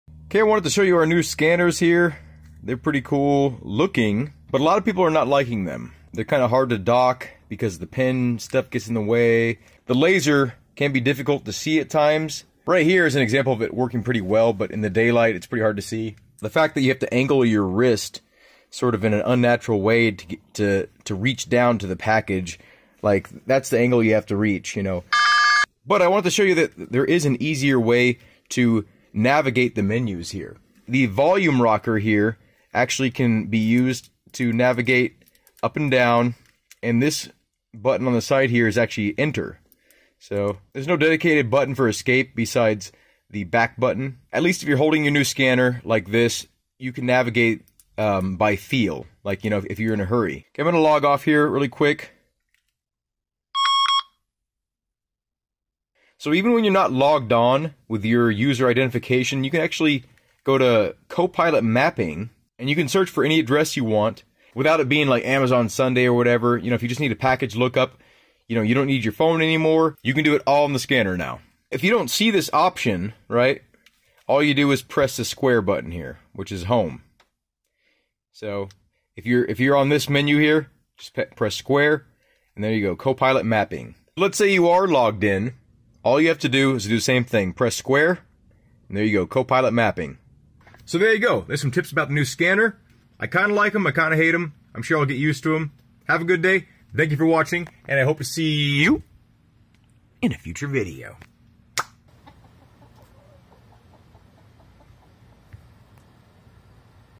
Hidden USPS New Scanner Features!!!
Tips:

1. Scan packages with the screen facing down, easier on the wrist.

2. Side buttons can control most navigation functions.

3. Home (Square) key takes you to a part of the device that allows for gps usage, no more draining your phone battery!

4. You have to remove strap in order to release battery pack.